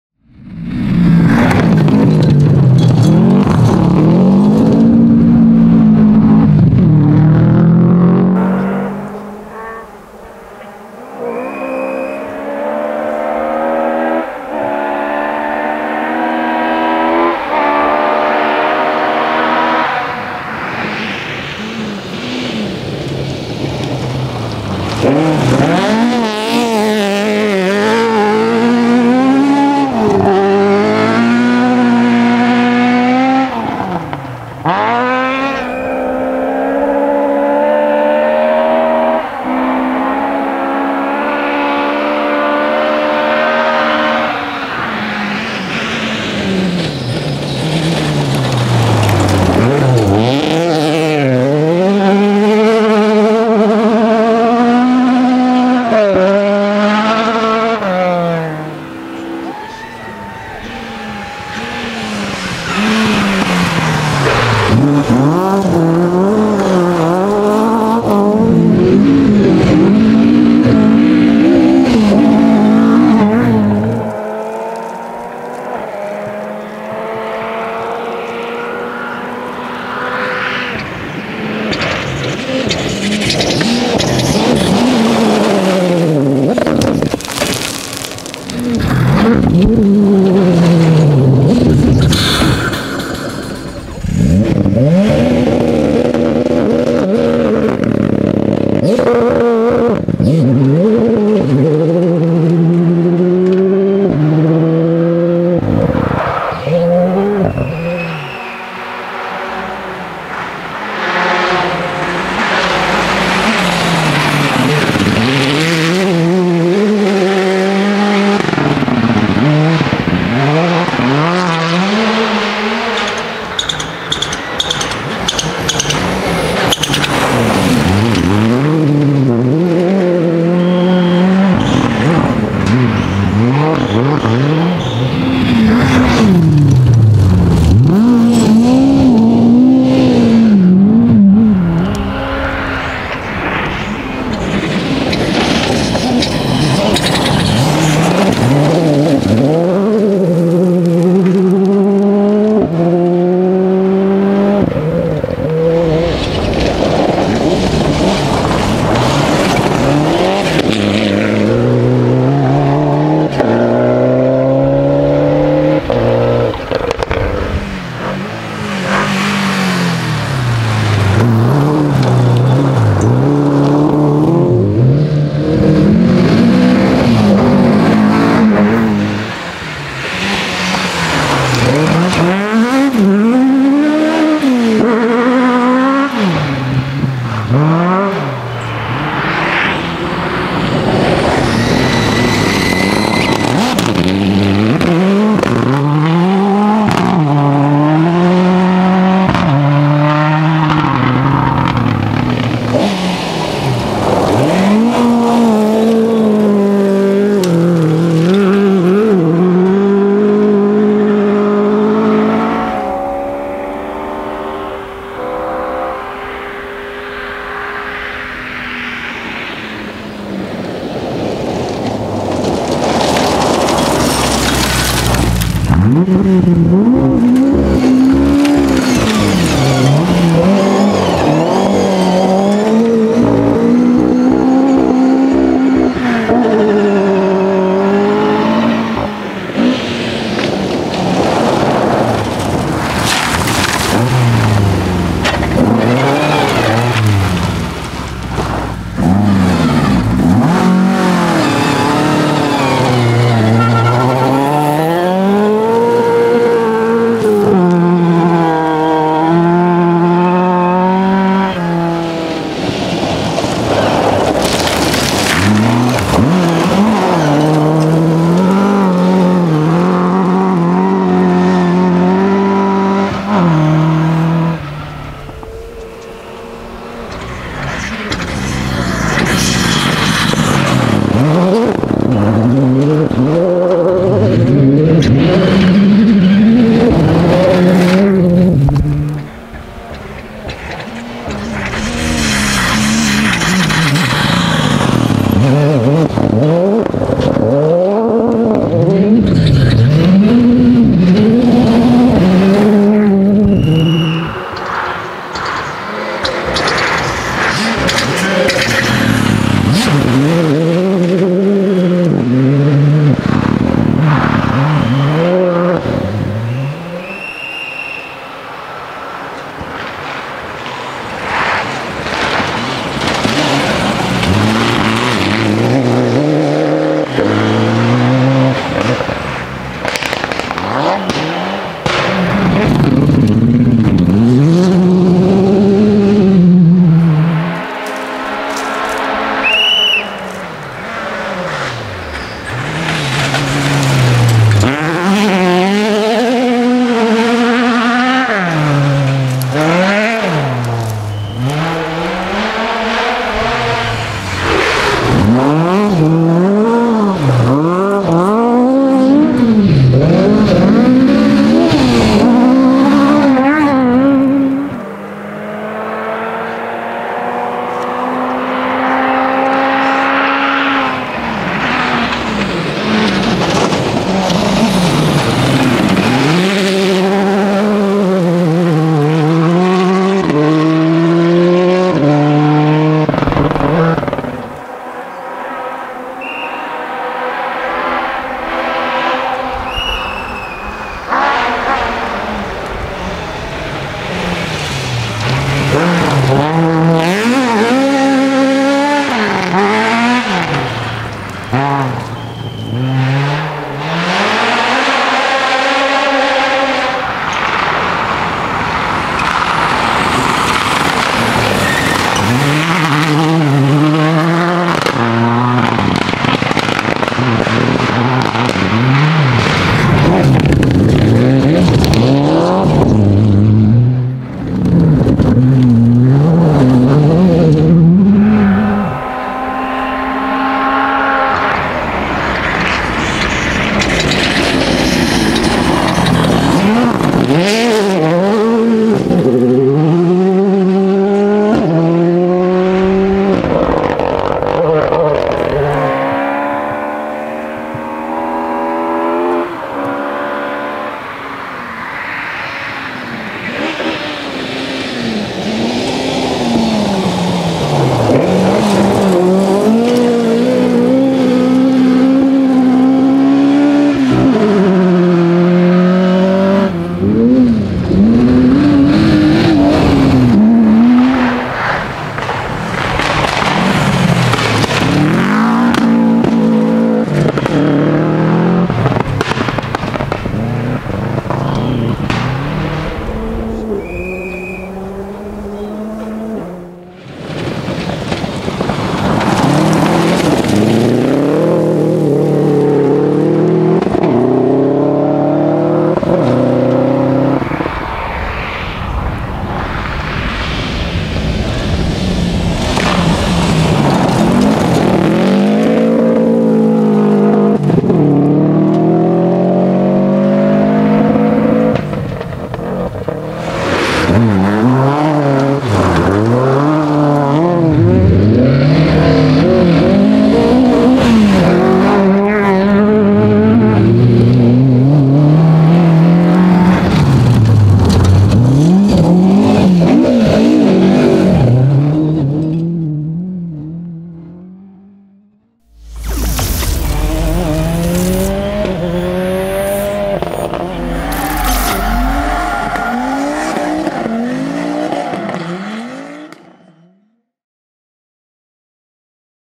RIPONIAN STAGES RALLY 2023
Highlight footage from the 2023 KTGreen Motorsport Riponian Rally Stages